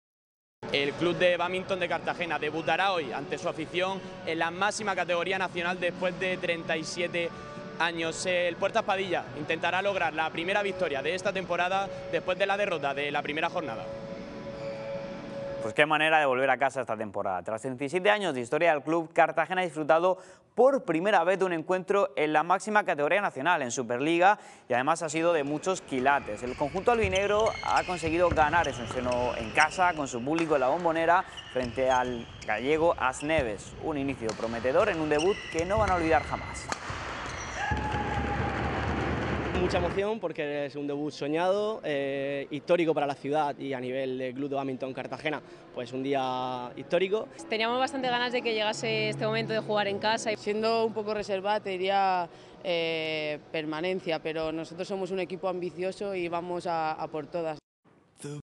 El BDM Cartagena debuta con victoria en casa
Que manera de volver a casa esta temporada. Tras 37 años de historia del club, Cartagena ha disfrutado por primera vez de un encuentro de la máxima categoría nacional. Y de muchos quilates, el conjunto albinegro ha conseguido ganar en su estreno en casa, con su público, en la bombonera frente a As Neves. Un inicio prometedor en un debut que no olvidarán jamás.